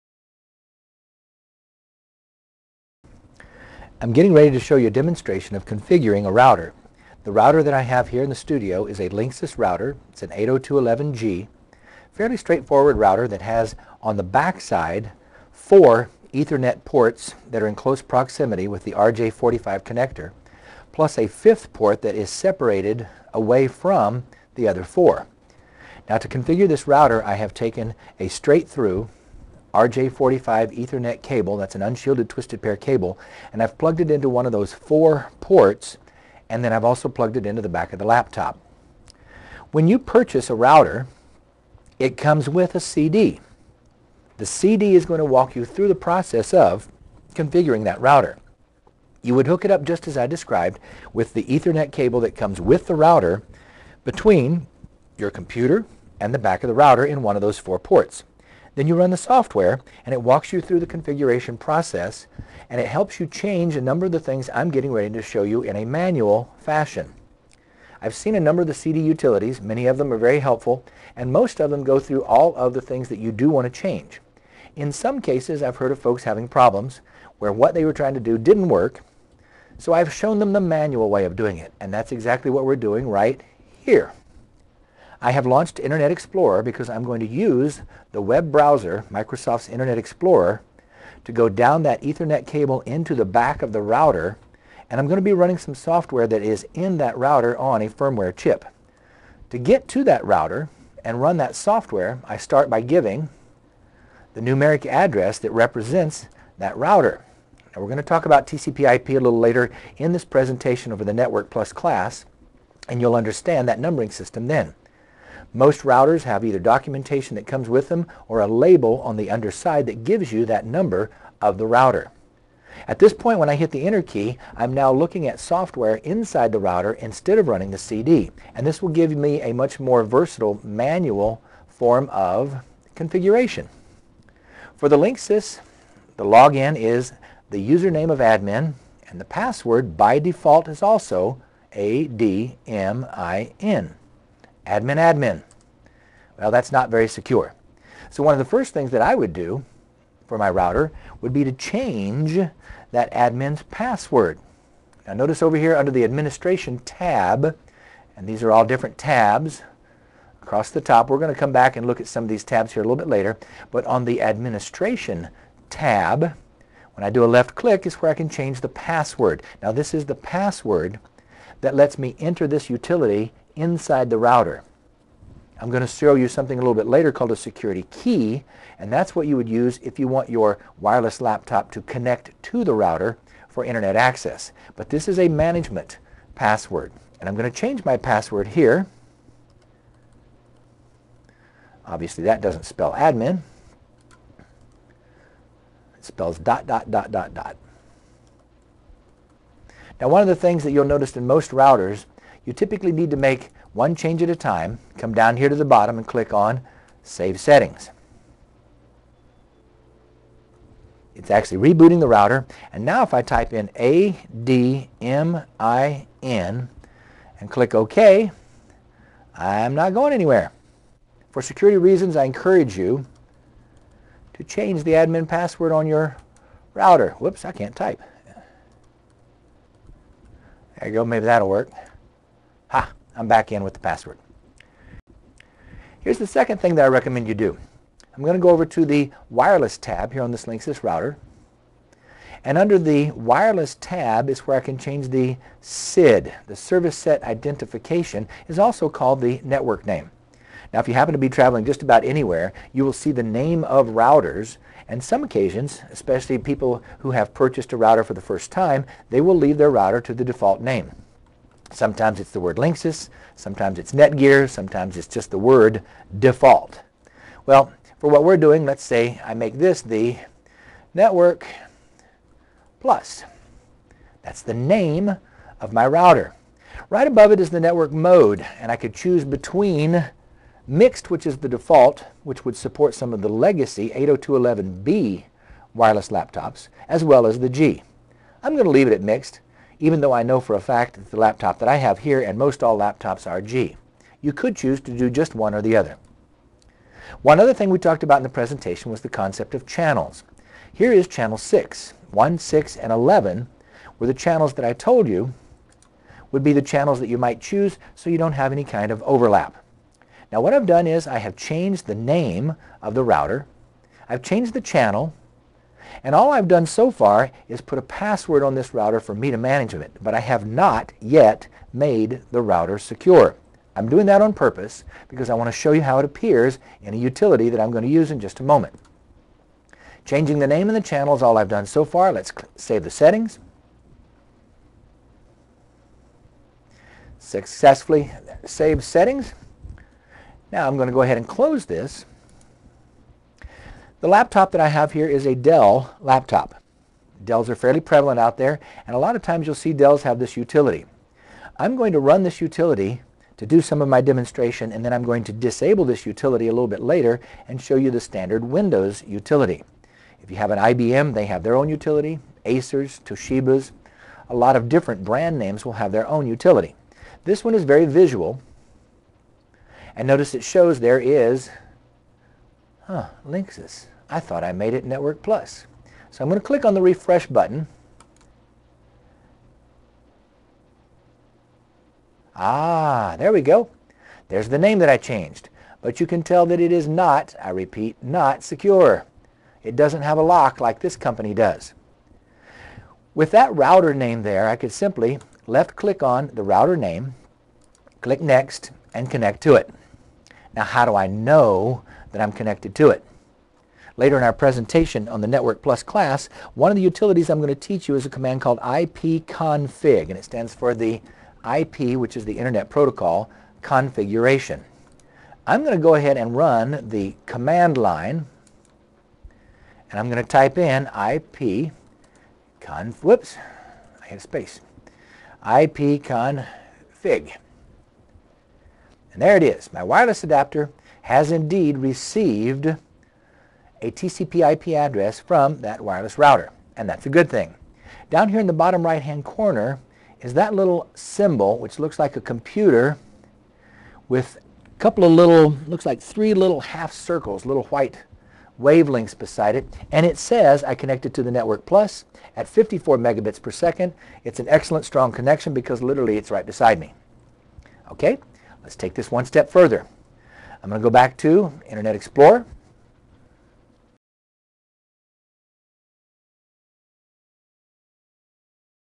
QuickCert CompTIA Network+ 2009: How to Configure a Wireless Network Router
Configuring a secure wireless router demonstration from QuickCert's CompTIA Network+ training course. Learn media and topologies, protocols and standards, network implementation and network support. for more information.